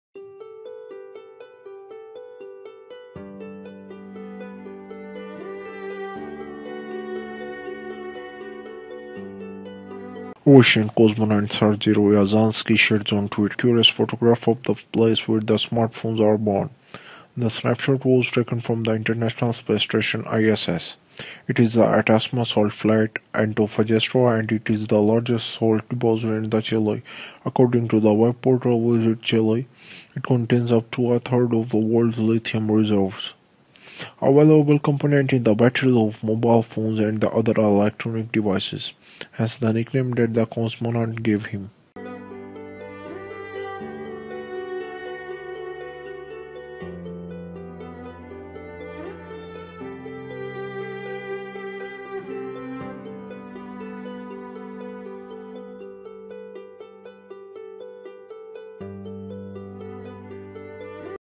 The Place Where Smartphones Are Born Is Seen From Space
A Russian cosmonaut takes a curious picture of a salt flat in Chile from the International Space Station.